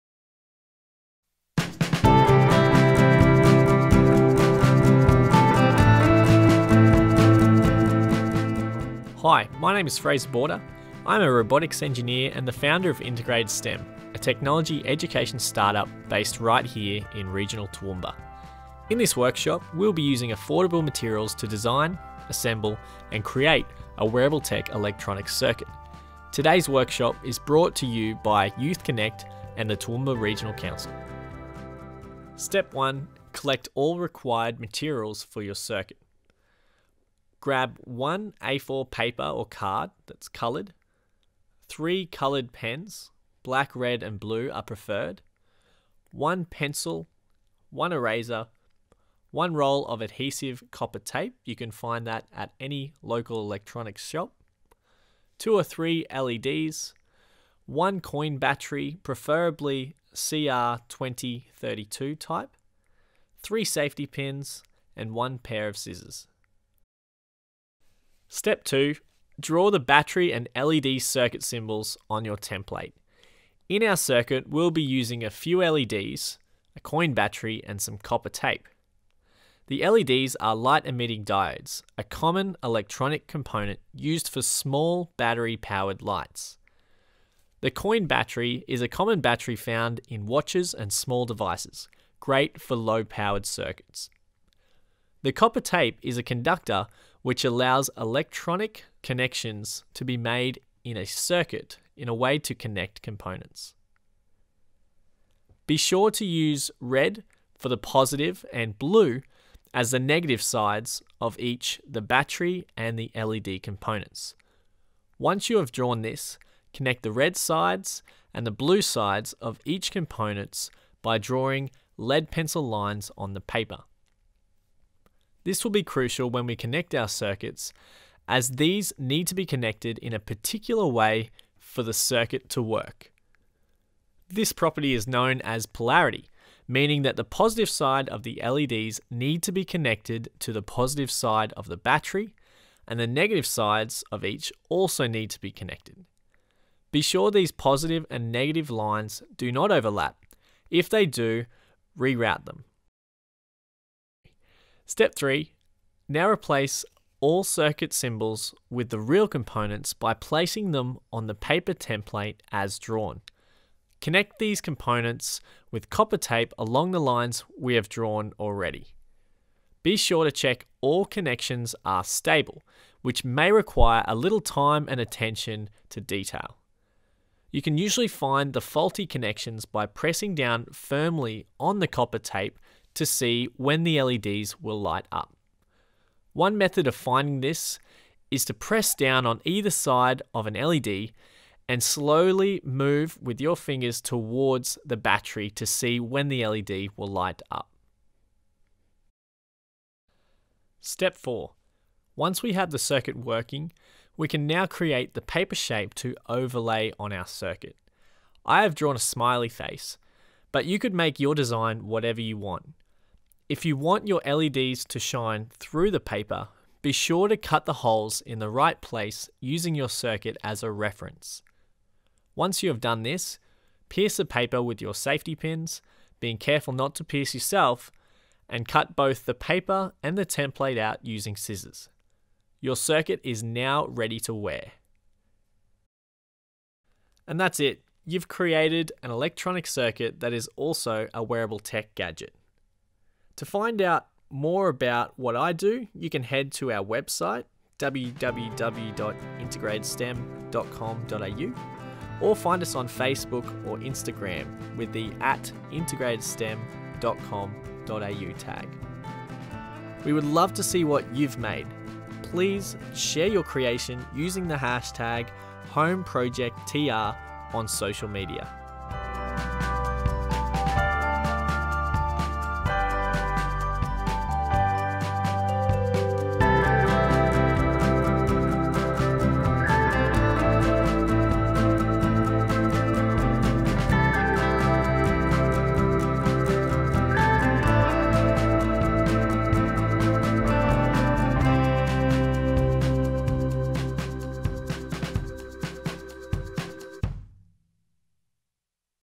The Home Project Ep. 17 – Wearable Circuit with Fraser Border
Got an interest in electronics and wearable tech? Or need some spooky Halloween ideas? Well, we have just the workshop for you!

Welcome to Episode 17 of The Home Project – free arts workshops to help keep you creative, active and connected.

Join Fraser from our friends at integratedSTEM for a crash course in creating your own wearable, battery-powered LEDs.

When you're finished, we'd love to see what you've made. You can share your creations with us using HomeProjectTR.

integratedSTEM are a locally-based team of young, passionate educators and engineers who offer practical, hands-on technology programs for students across Australia. Check out their awesome work on –